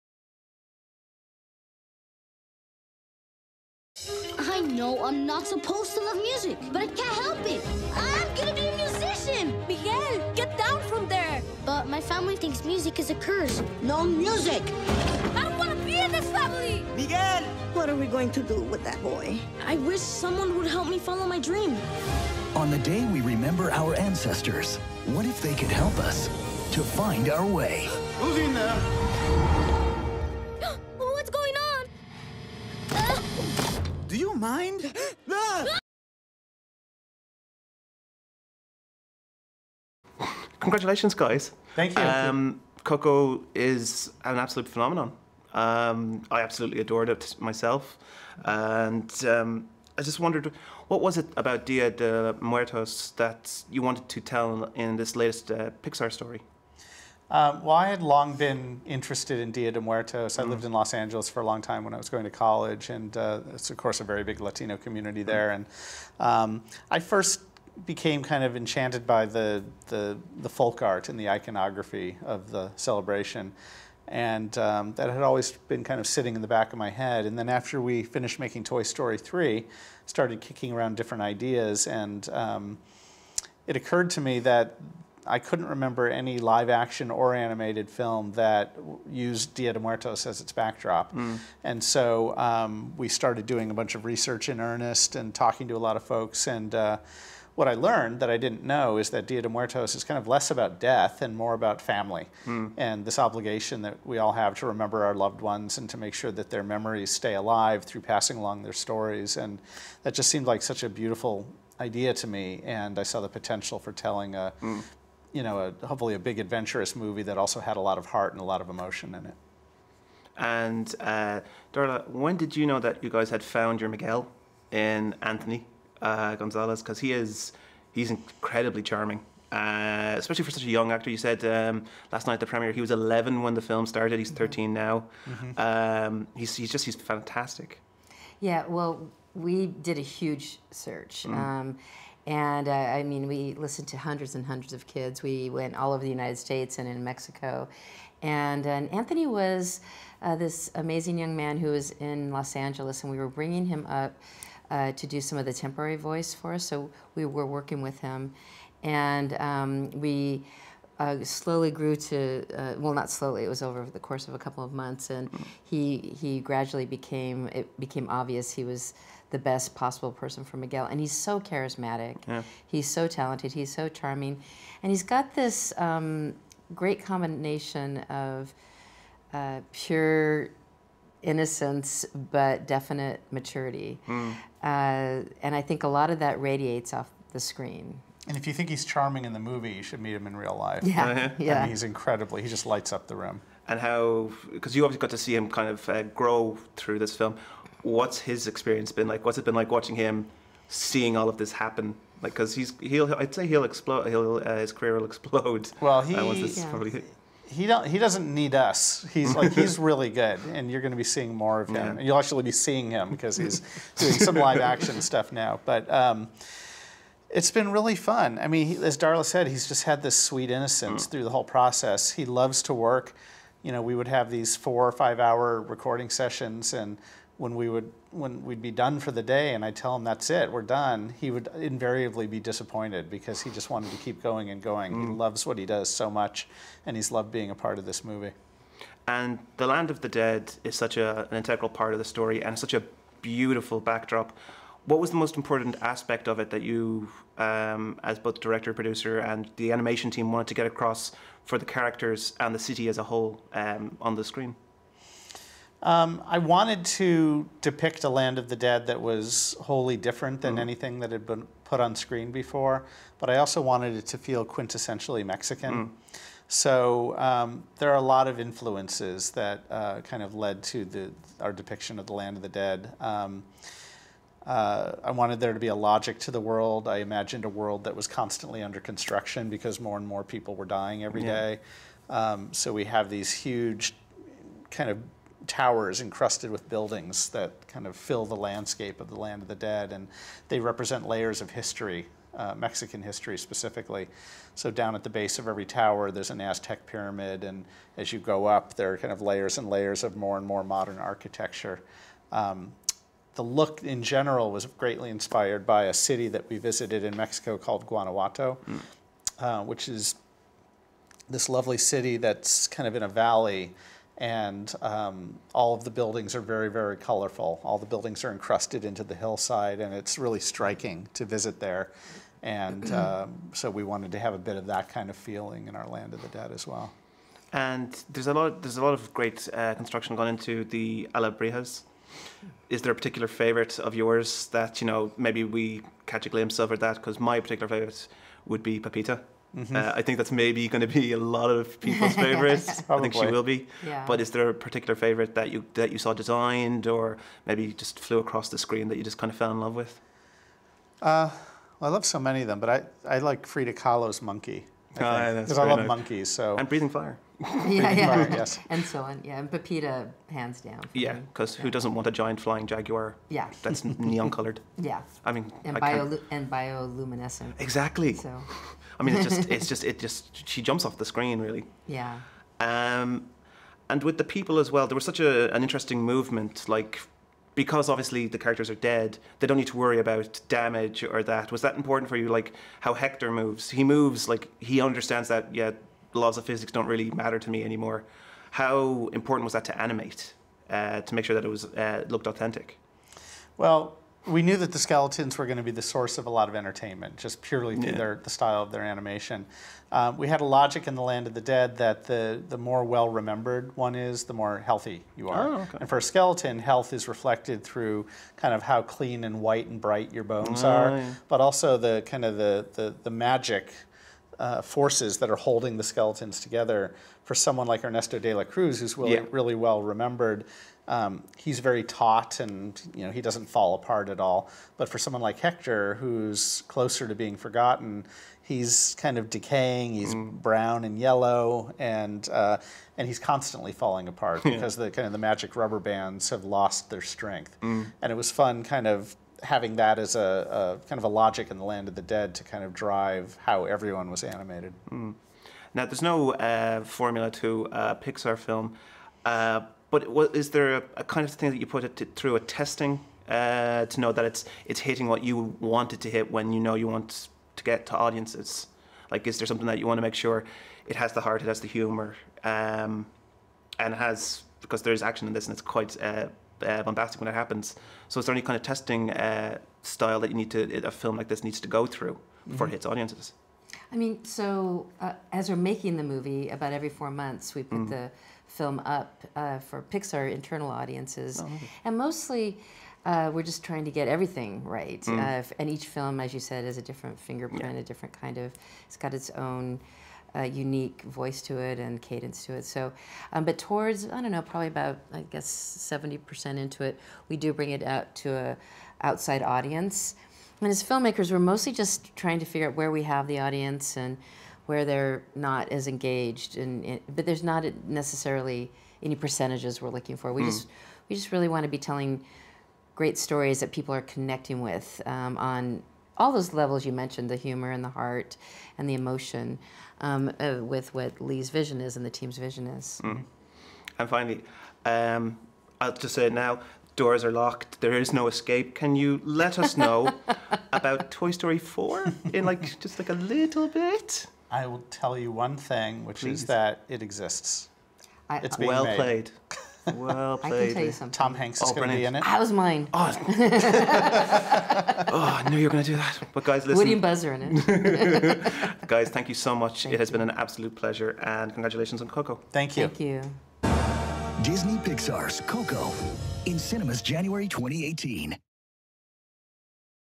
Coco Interview - Director Lee Unkrich and Producer Darla K. Anderson
Scannain talks Coco with Director Lee Unkrich and Producer Darla K  Anderson. Out now in Irish cinemas.